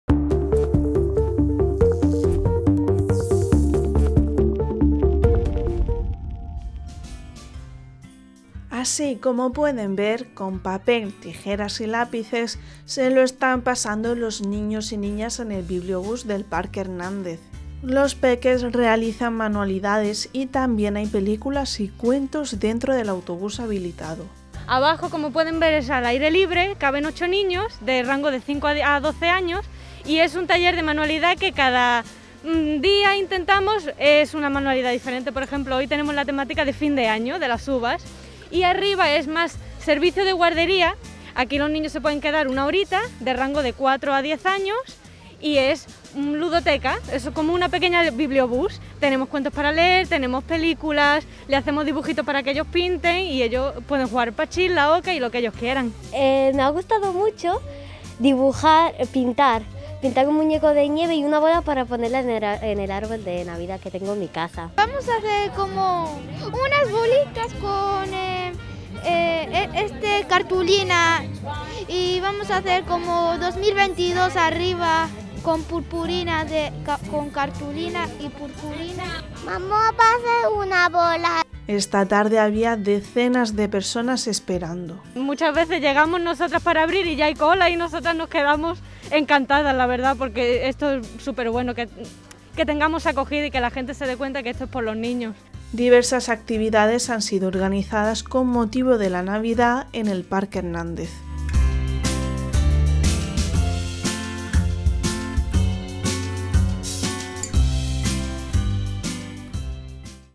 Papel, tijeras y mucha diversión en el bibliobús del parque Hernández en Melilla
Así como pueden ver, con papel, tijeras y lápices, se lo están pasando los niños y niñas en el bibliobús del parque Hernández.

Redacción y locución: Alicia Martínez